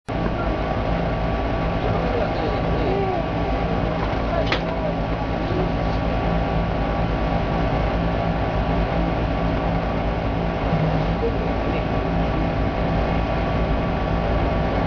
Kyoto Daimonji
On August 16, peoplr in Kyoto send out spirits of their ancestors with the designed fires over the mountains.